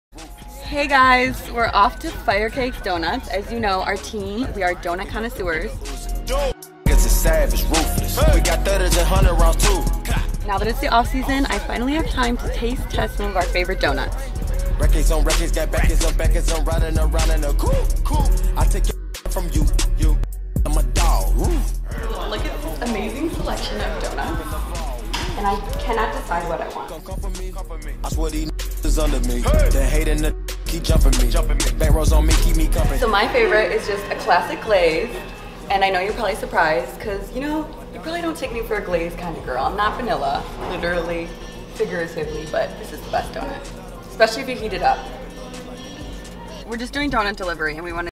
Sarah Gorden National Doughnut Day
It's National Doughnut Day! Check out our favorite defender and doughnut queen, Sarah Gorden and her obsessed team the Chicago Red Stars.